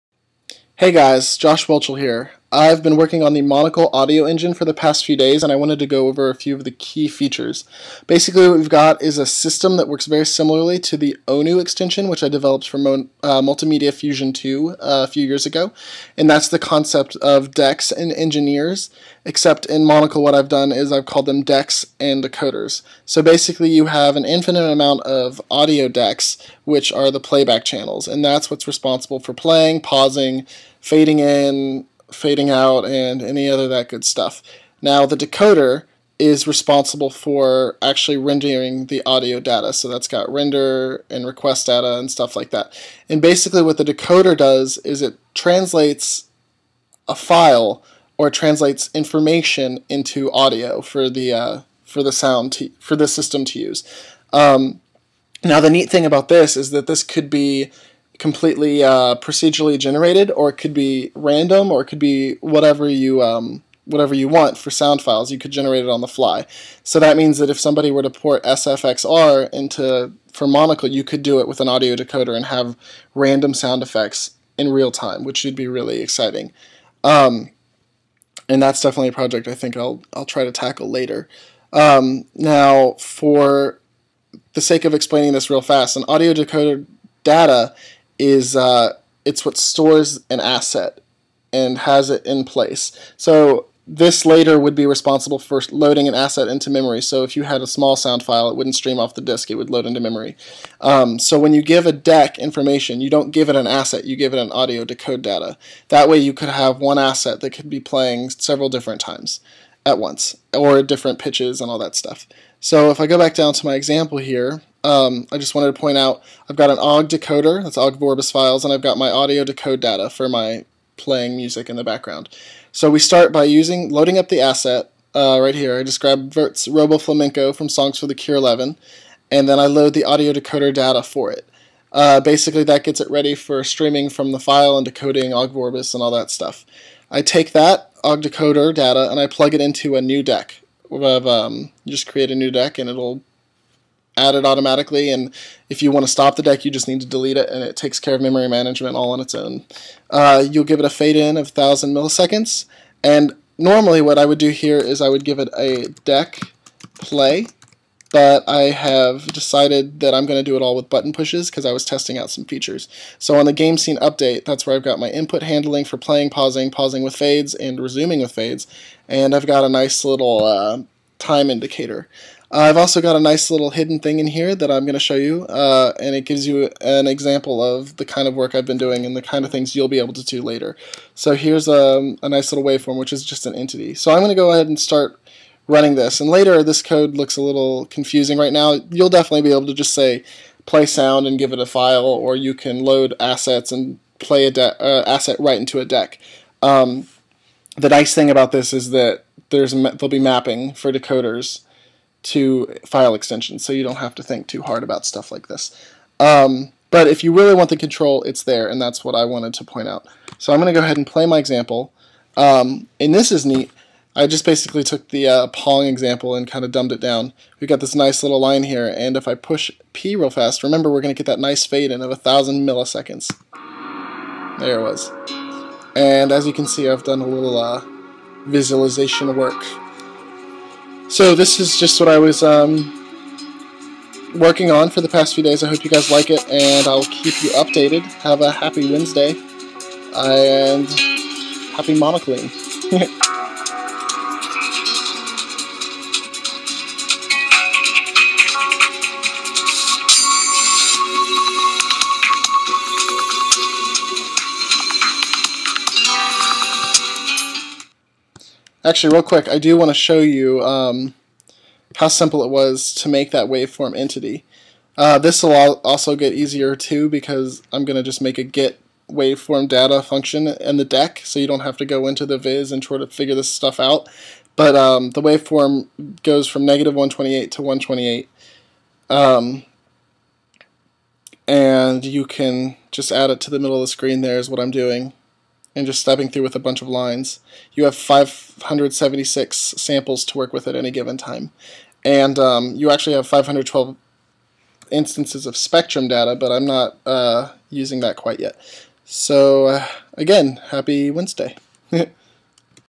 Monocle: Audio Engine Demo 1
A simple demo for the audio engine in Monocle which will... blow your mind.